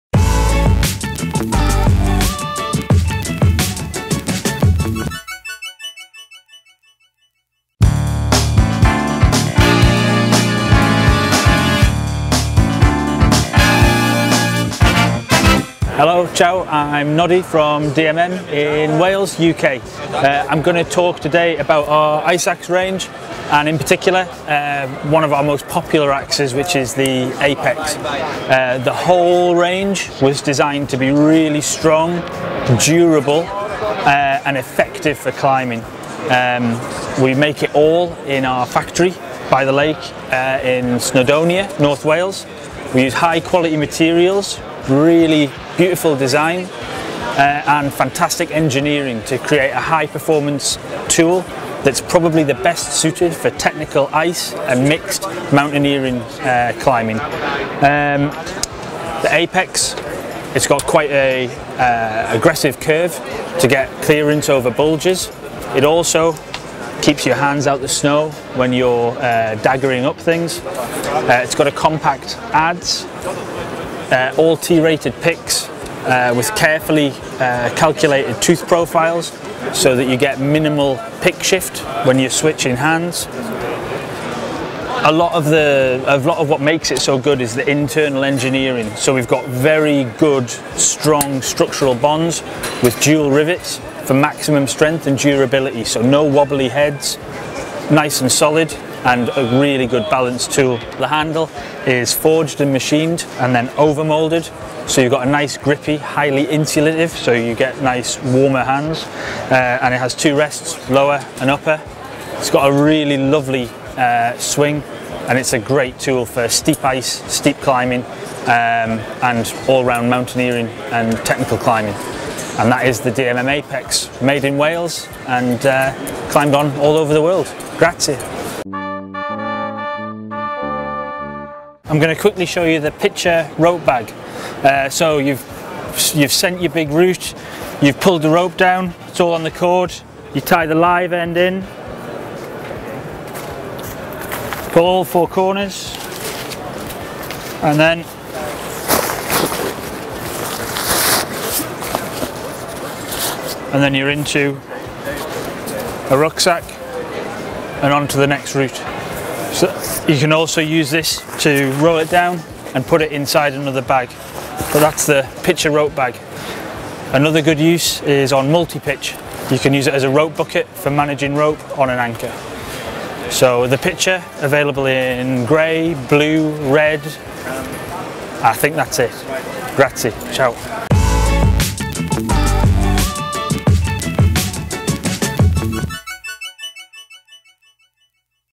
Oliunìd @ ISPO 2018 - Ep. 1R: DMM Apex e zaino Pitcher
ISPO Monaco 2018. Notty di DMM ci spiega perché la piccozza Apex è così efficace sul ghiaccio e sul misto. Ci mostra anche il comodissimo zaino Pitcher, ottimo sulle vie lunghe per organizzare la corda.